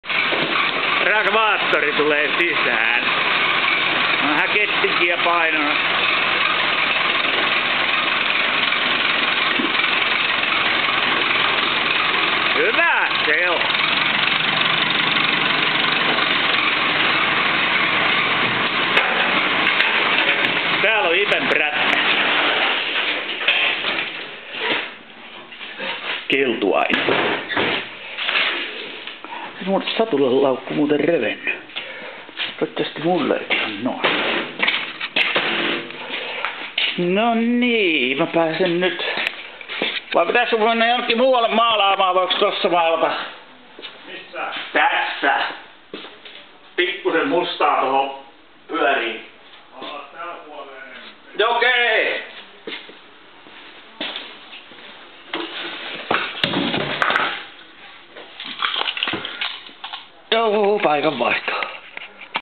Peräkärryn akselin maalausta
Meninkin sitten toiseen paikkaan maalaamaan ette taukotilaan tuu käryä